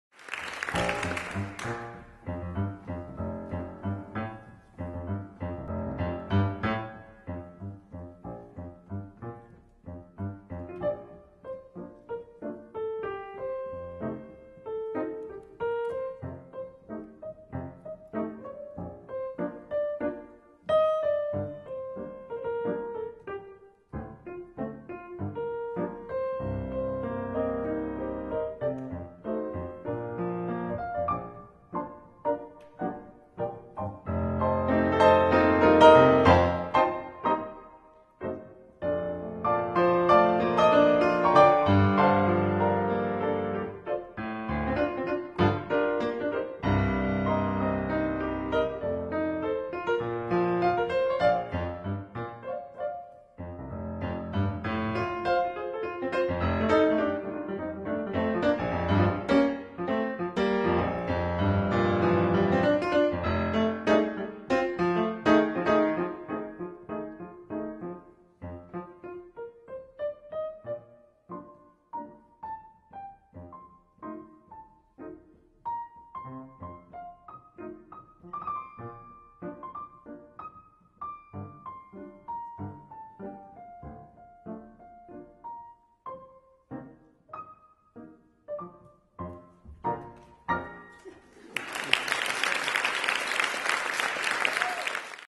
Jauniešu mūzikas festivāla (ESTLAT) goda viesis - Raimonds Pauls
Goda viesis - Raimonds Pauls. Izpildīja divas latviešu tautasdziesmu apdares